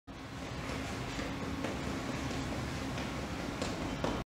Fast Feet on Ladder - Running Drills
Fast Feet Drills for running speed and cadence improvement.  See the complete article on Cadence Drills